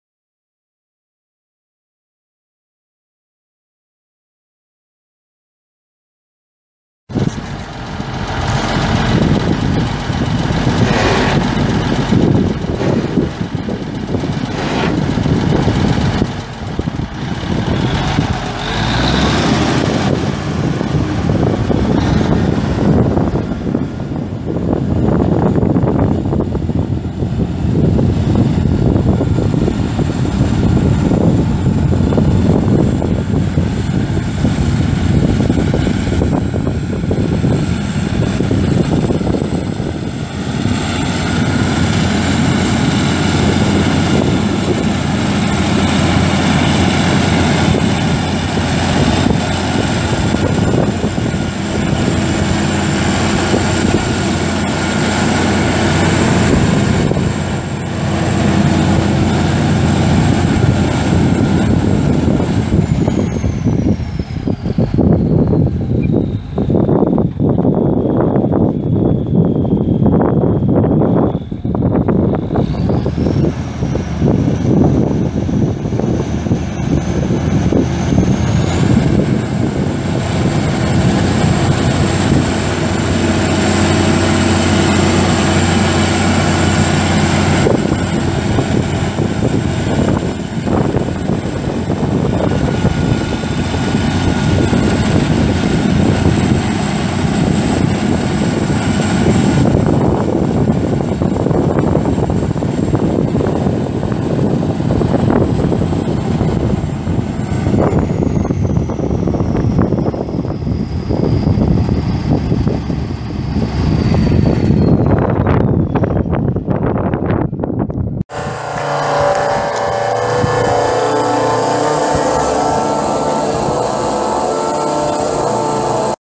1995 Ford 9680 4WD tractor for sale | no-reserve Internet auction September 20, 2017
View a detailed listing of this item and place bids

1995 Ford 9680 4WD tractor
7,859 hours on meter
Cummins NTA-855-A 14.0L six cylinder turbo diesel engine
350 HP
Manual transmission
12F-4R gears
AM/FM
20.8R42 tires
Serial D103028
It's located in Channing, TX, and we're selling it to the highest bidder regardless of price.